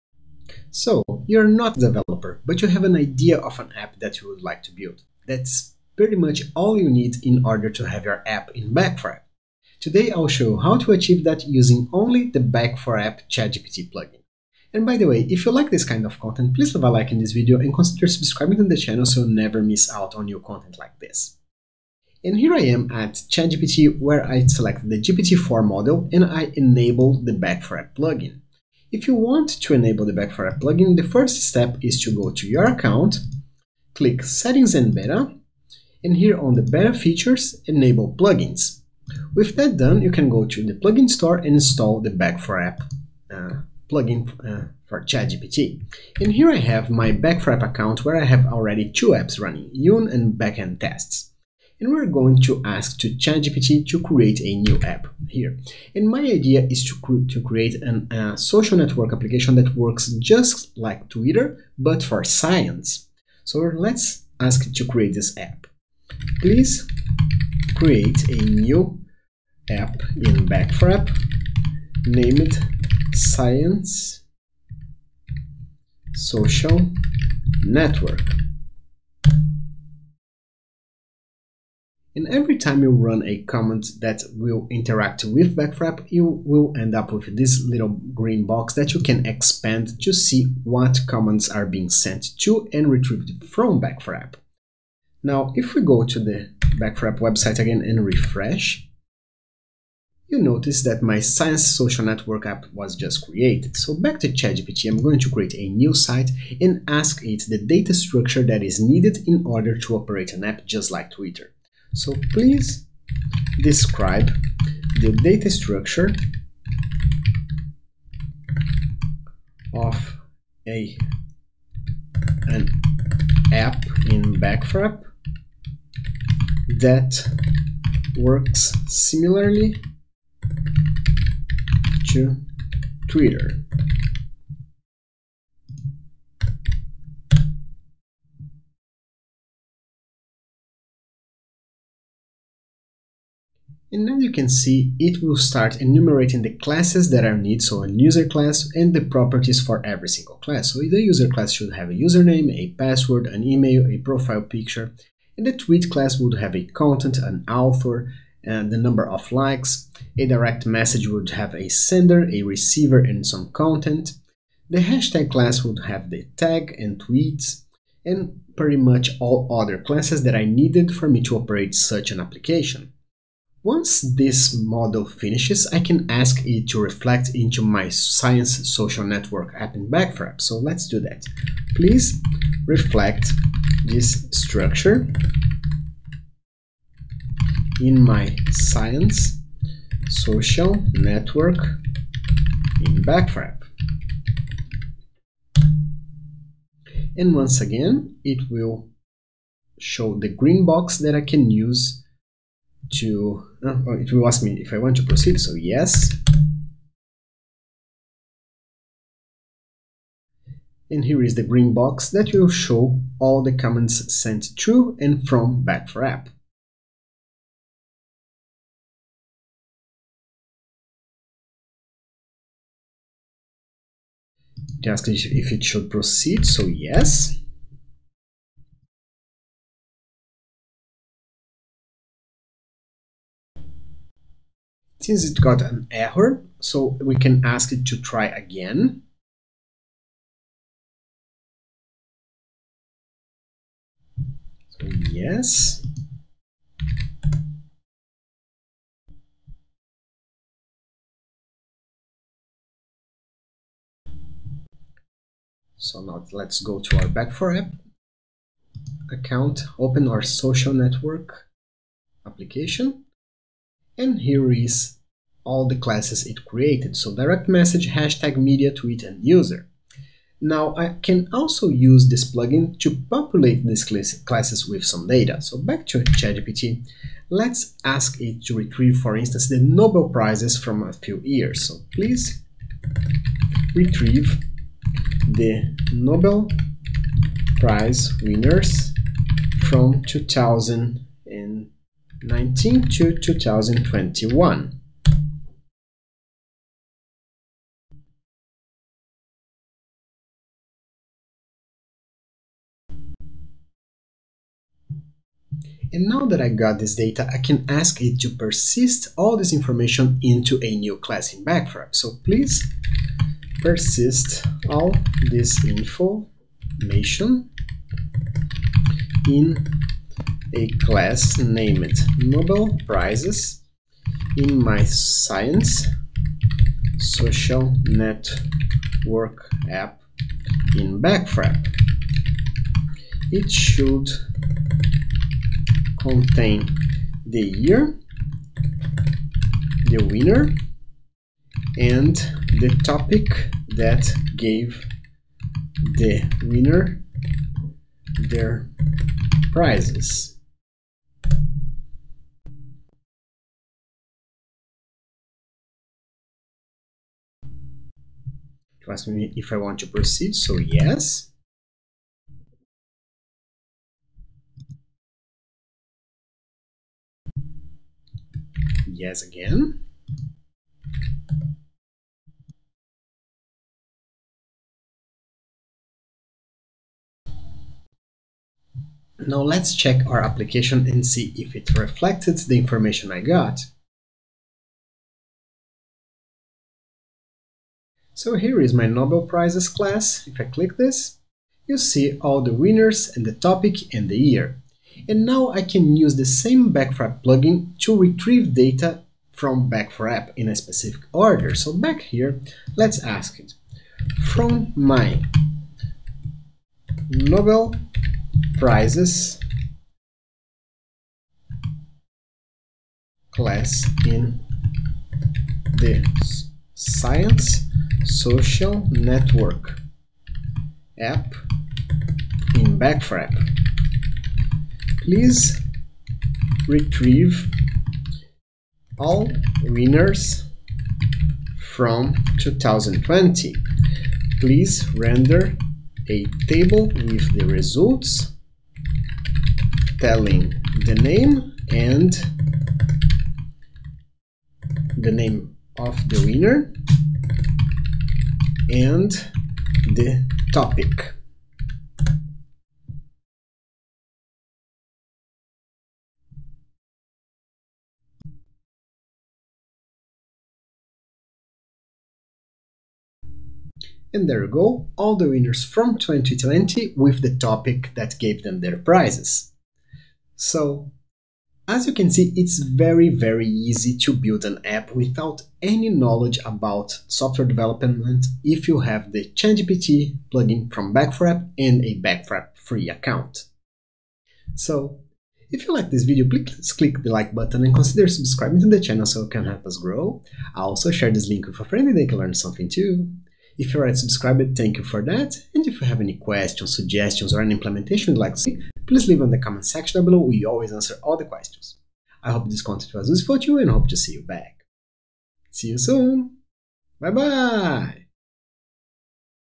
Build an App with Prompts on ChatGPT
If you're not a developer but need to develop an app, we got you covered!

This video you'll see how to develop an app if you're not a developer by using the @Back4App ChatGPT plugin (yes it's FREE!).

Back4App’s ChatGPT plugin reaffirms  commitment to delivering cutting-edge technology to make development simpler, quicker, and more effective, even if you are not a Developer.

The ChatGPT Plugin allows even those that can't code a single line of code, to create applications, data models, store information, and perform analysis using everyday language and prompts.